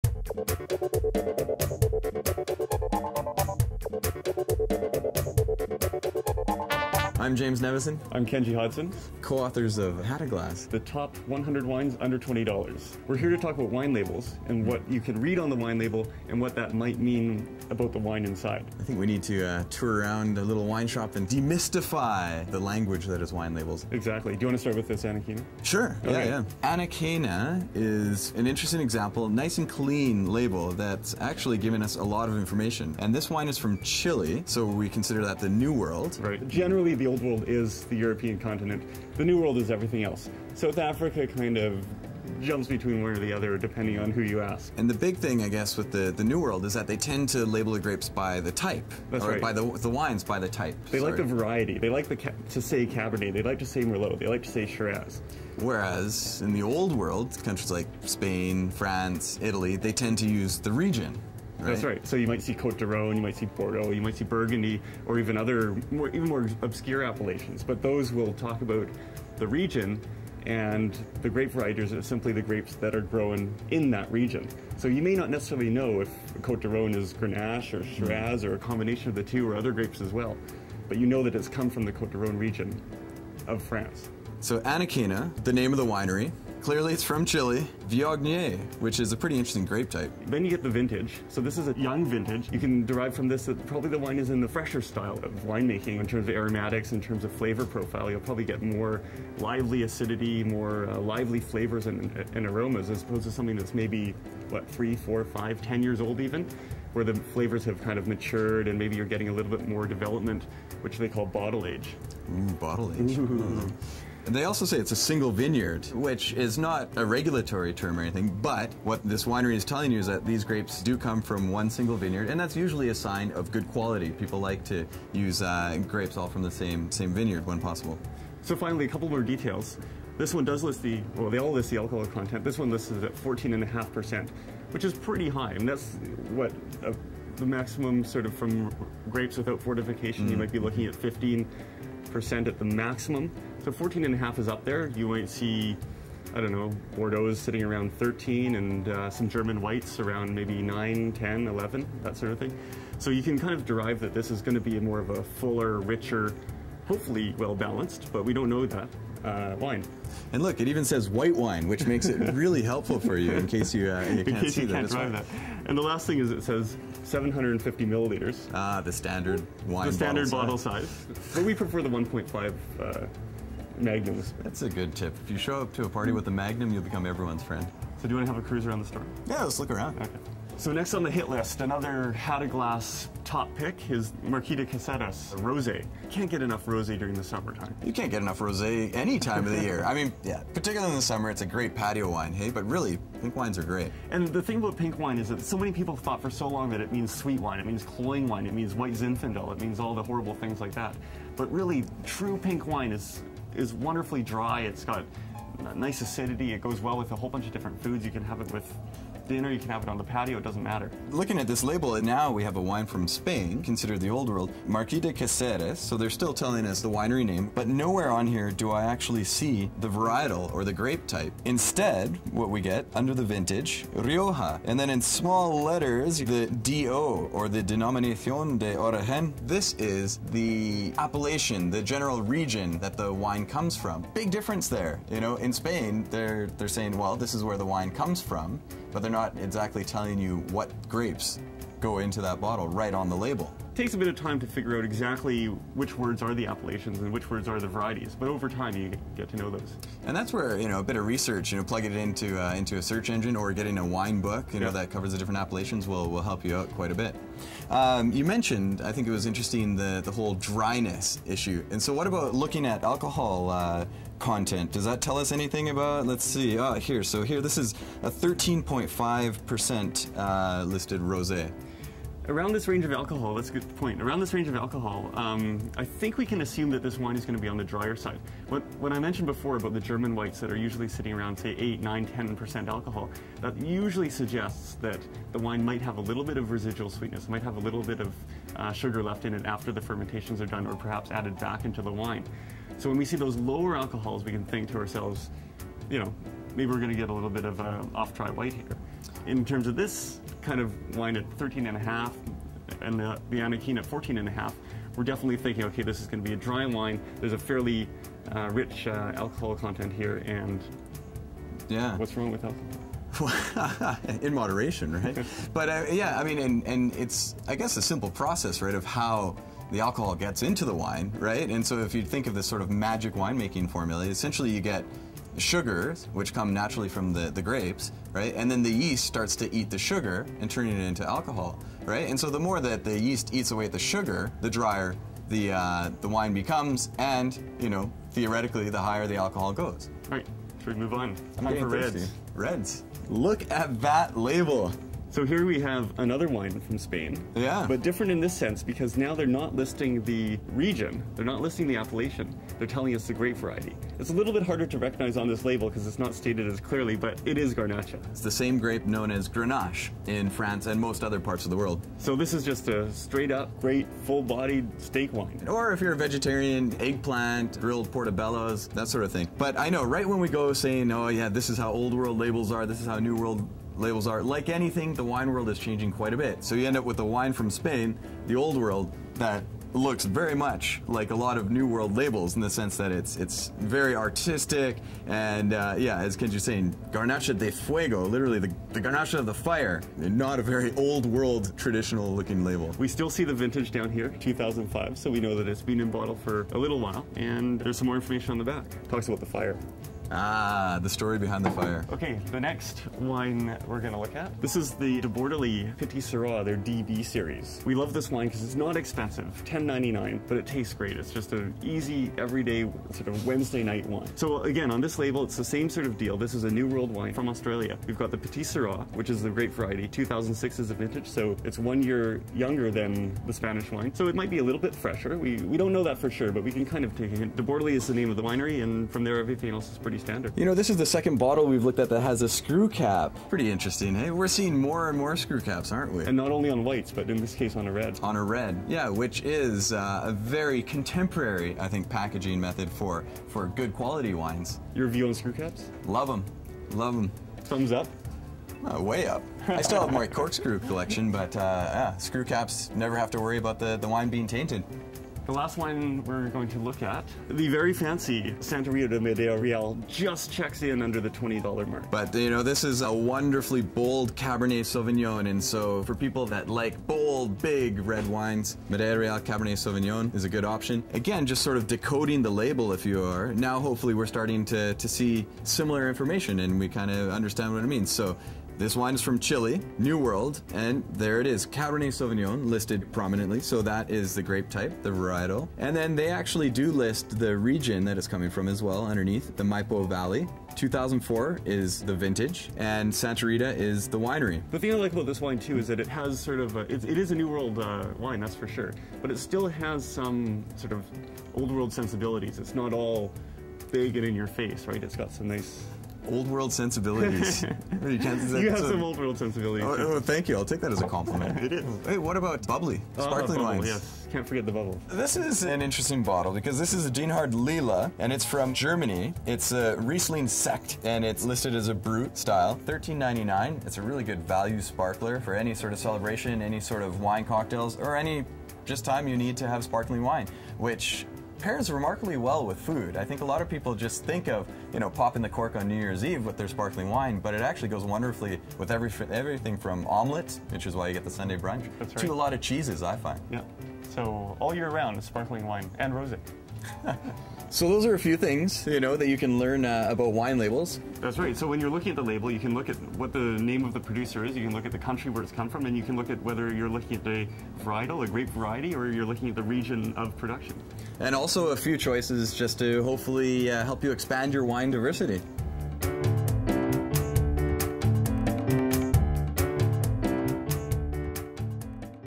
Demystifying Wine Labels
James Nevison and Kenji Hodgson talk about how to read wine labels and how to understand things like old world / new world, grape variety, wine type, alcohol percentage, vintage etc.